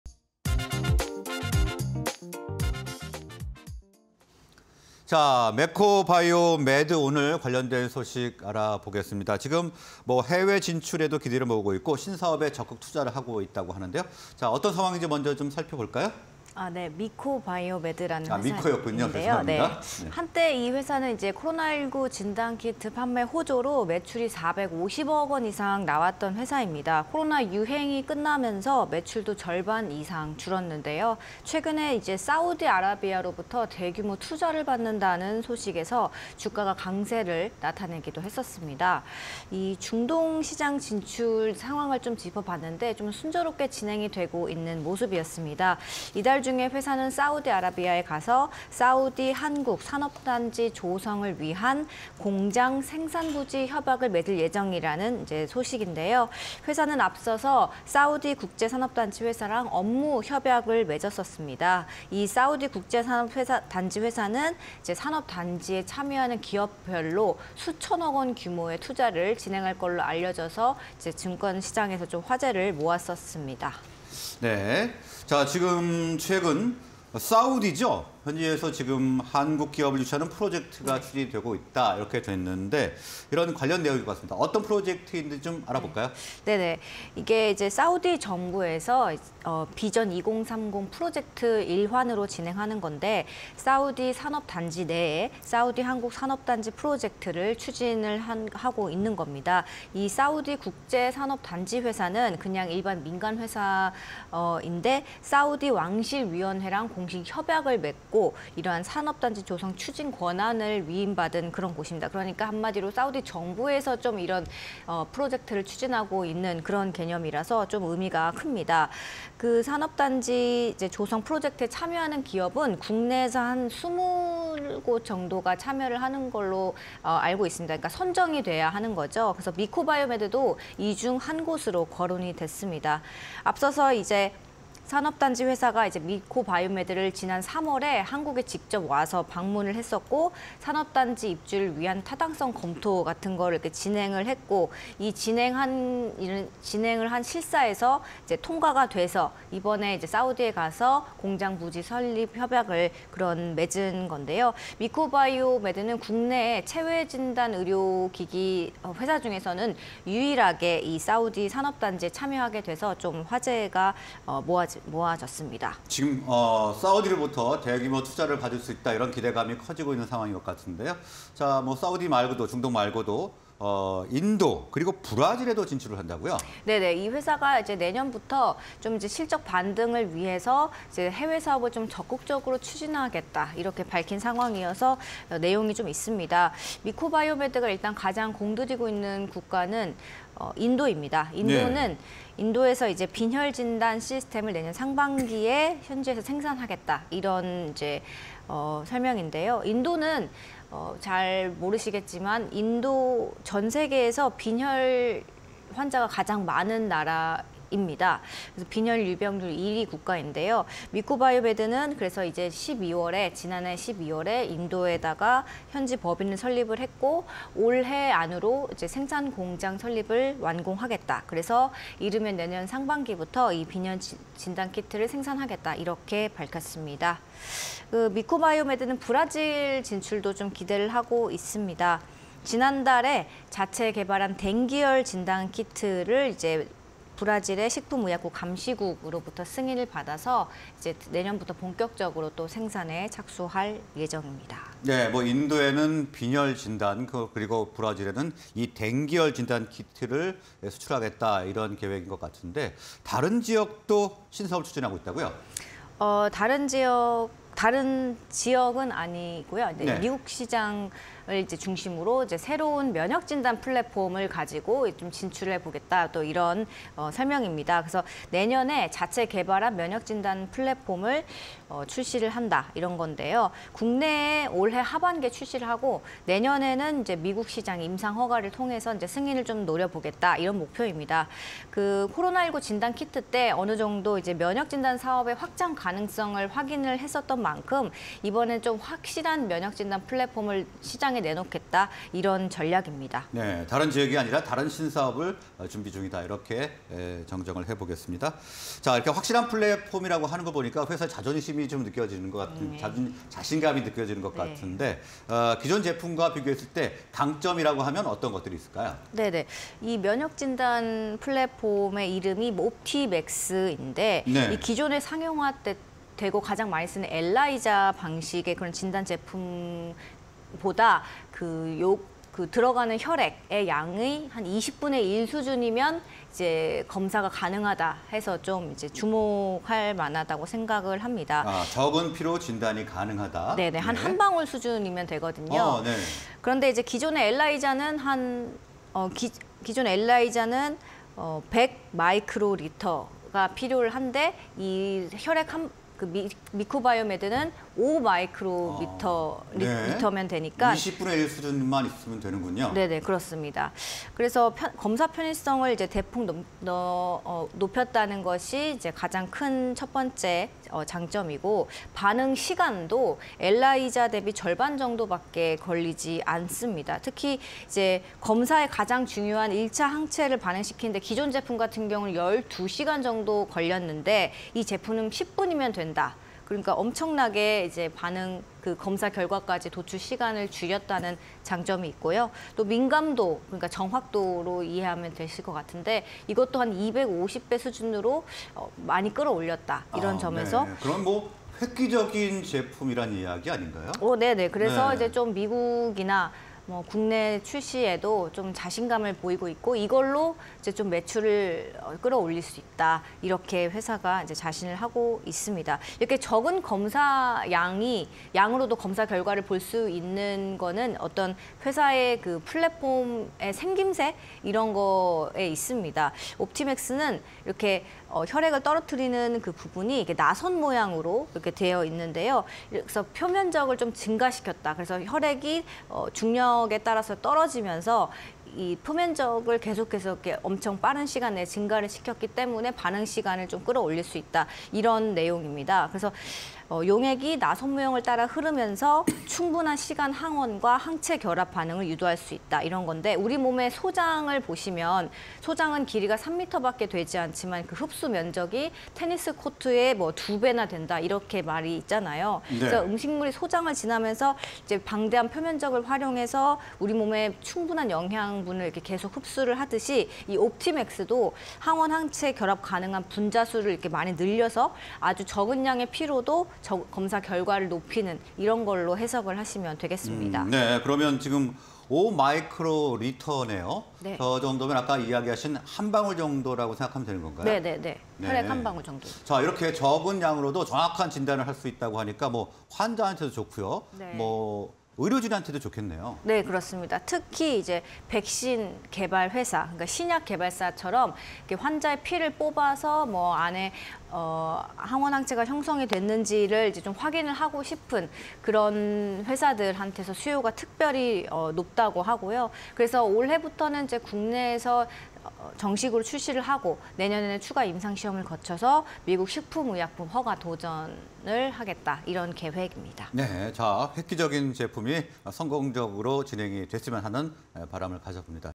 미코바이오메드, 해외 사업 확장... 내년 실적 반등 노리나?_기자들의 백브리핑 (20231108)
Q. 미코바이오메드, 해외 진출 속도 내나?
Q. 미코바이오메드, SIIVC 투자 주목... 중동 시장 공략?
Q. 미코바이오메드, 인도·브라질 시장 진출 가시화?
Q. '자체 개발' 면역진단 플랫폼, 기존 제품 대비 강점은?
Q. 미코바이오메드 '옵티맥스' 경쟁력은?